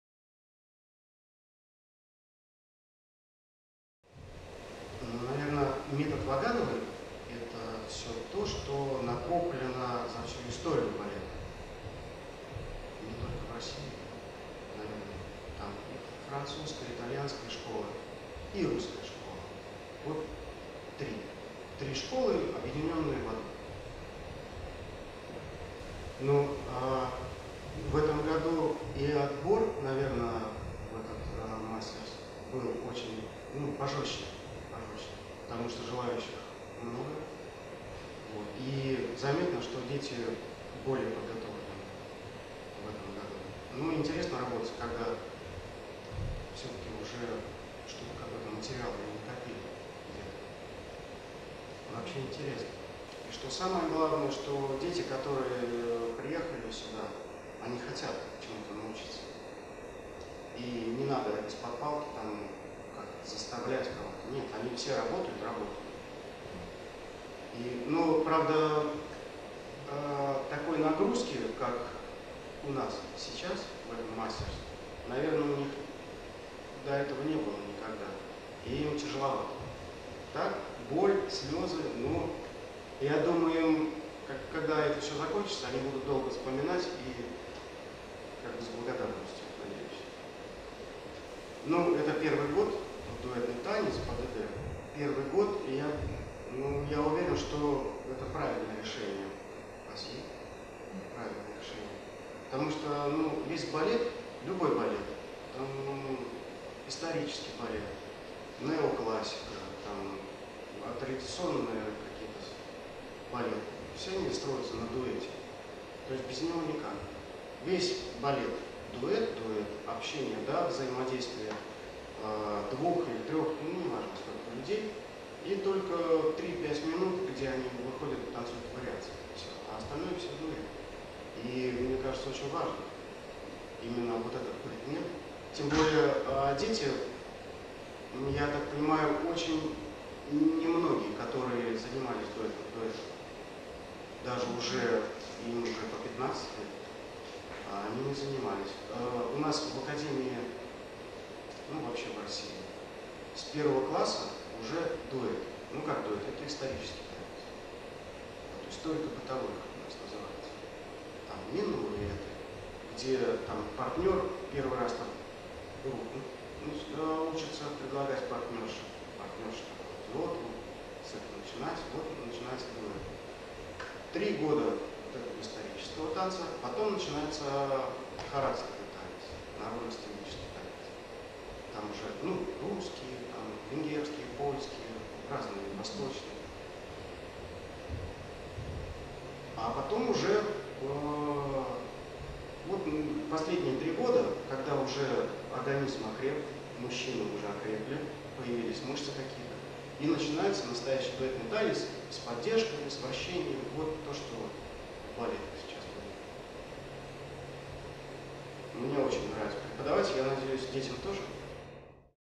Interview with Dmitry Shevtsov - actual teacher of Eifman Ballet School / Russian Masters Ballet
Alicante, Spain
10-30 July 2017

World reference ballet intensive of pure Vaganova method.